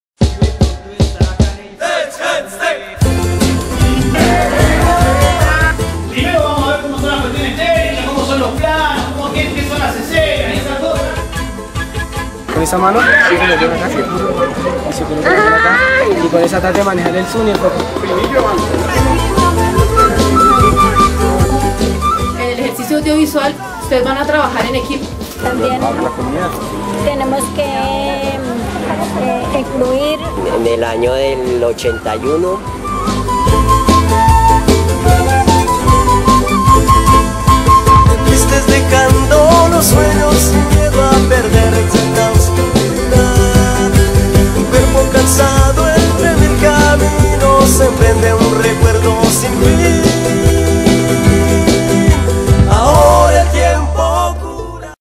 Promocional talleres Festival Ojo al Sancocho cva 025 2012
Participa en los talleres del 5 Festival Internacional de cine y video alternativo y comunitario Ojo al Sancocho. Mas info en: ojoalsancocho.org/